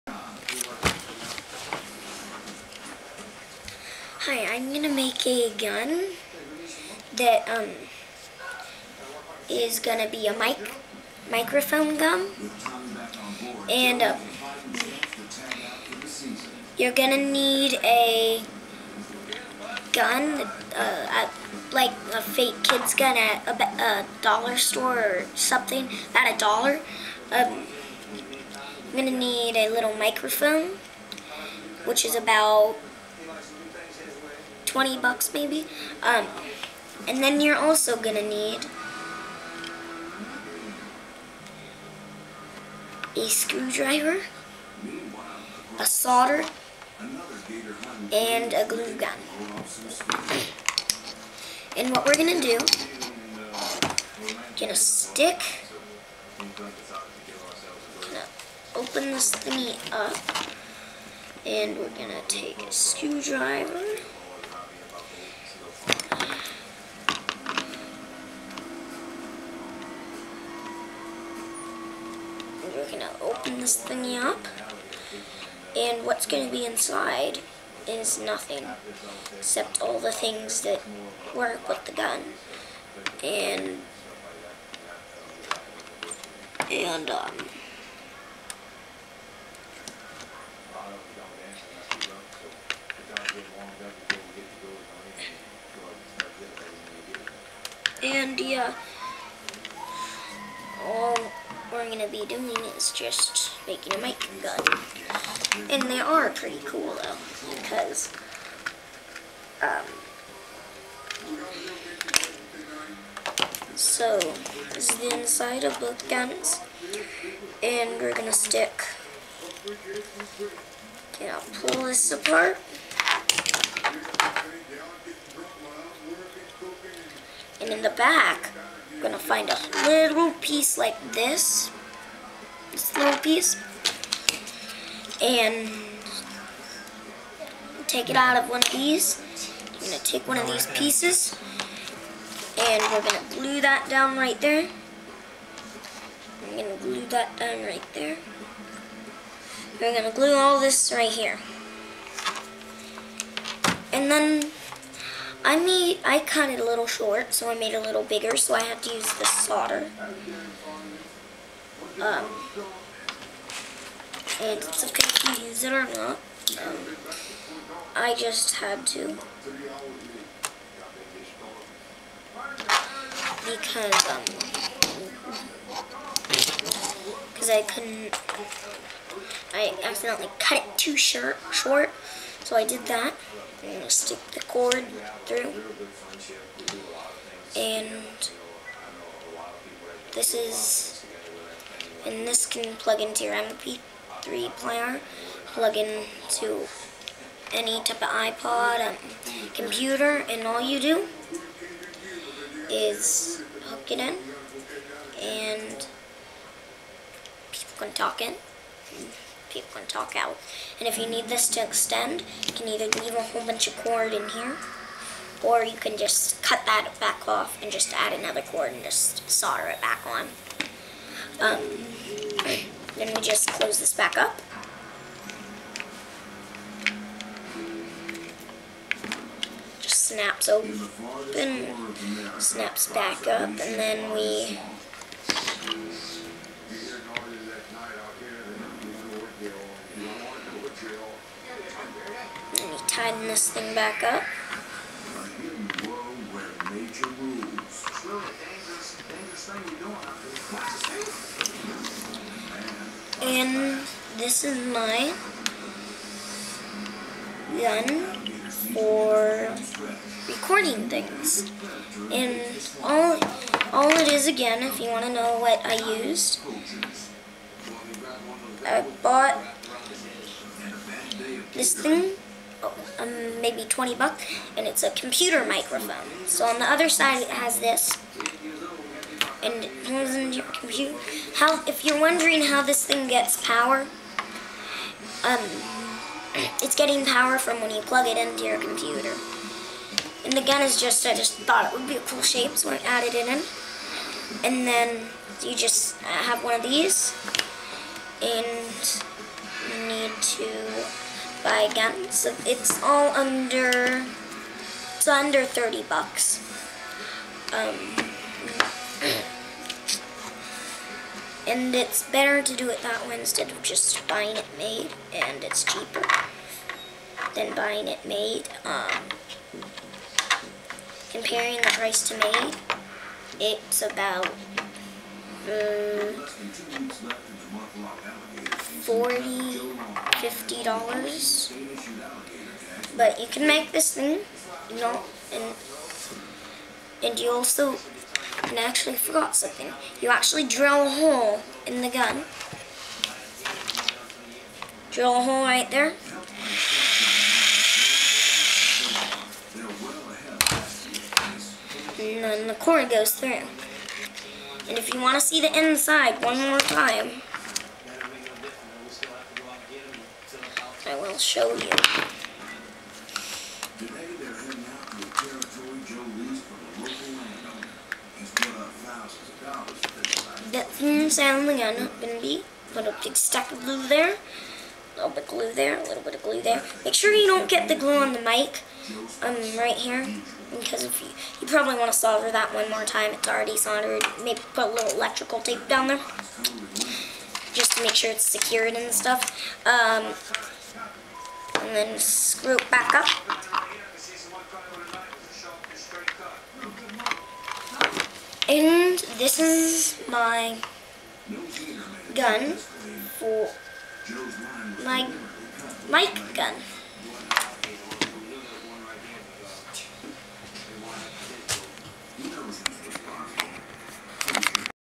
Microphone gun
Just a covert "microphone gun" I made with a cheap microphone, a glue gun, solder, and a toy gun.